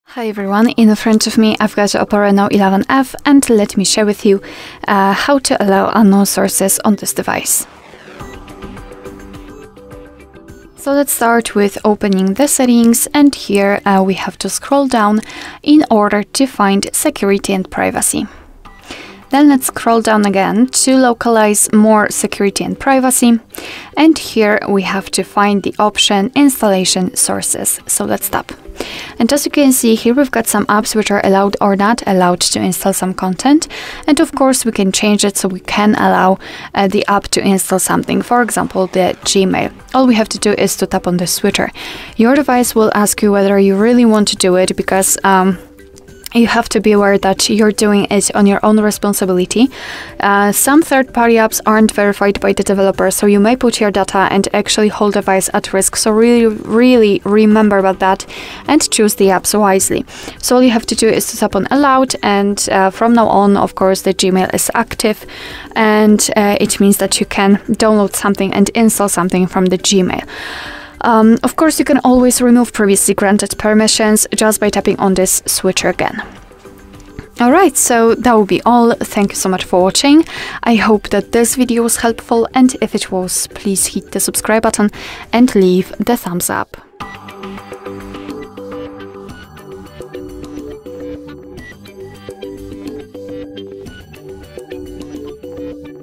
How to Allow Unknown Sources on Oppo Reno 11F?
Unlock more possibilities with our straightforward guide. Learn how to enable Unknown Sources on your Oppo Reno 11F to install apps from sources outside the Google Play Store. Say goodbye to app restrictions and hello to a world of diverse applications!

How to access security settings on Oppo Reno 11F?
What are the steps to allow Unknown Sources on Oppo Reno 11F?
How to ensure app security while installing from Unknown Sources on Oppo Reno 11F?